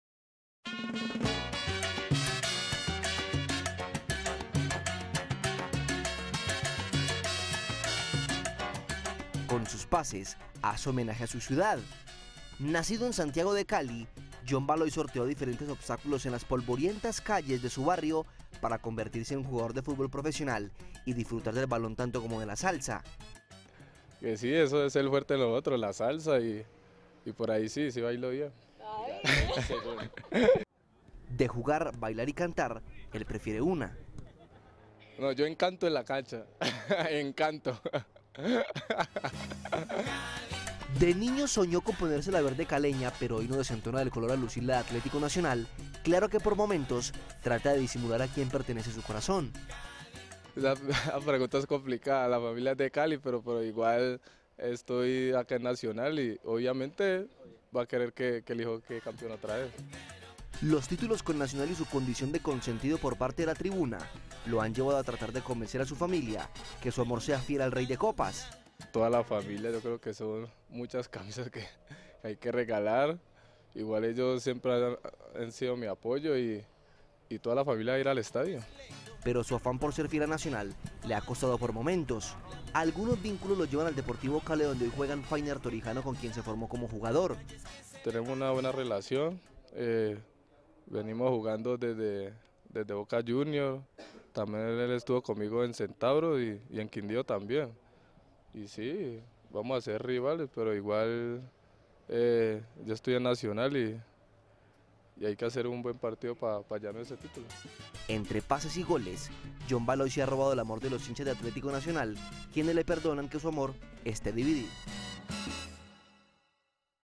John Valoy en busca de la gloria, contra el equipo de sus amores
Varias historias se tejen en torno al encuentro de este domingo a las 5:30 de la tarde. Una de ellas es la de John Valoy, jugador vallecaucano quien divide por estos días el amor de sus amores con el que le ha dado títulos como profesional.